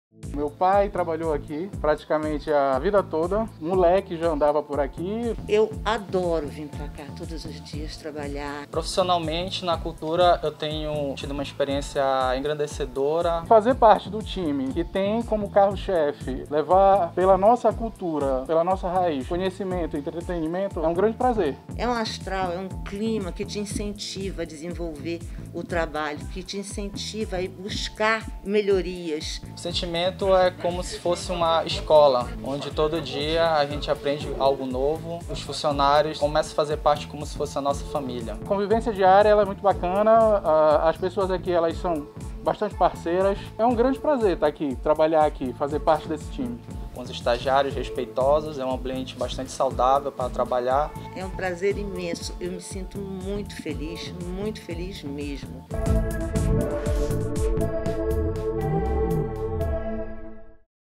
Especial | TV Cultura 34 anos
31 de março de 2021 a TV Cultura completou 34 anos. Em um momento tão difícil, tivemos que nos readaptar para levar a notícia até você, telespectador. Sempre para lhe deixar bem informado. Nossos colaboradores são fundamentais nesse papel e, como forma de homenageá-los, eles puderam contar um pouco do que a TV representa para cada um deles.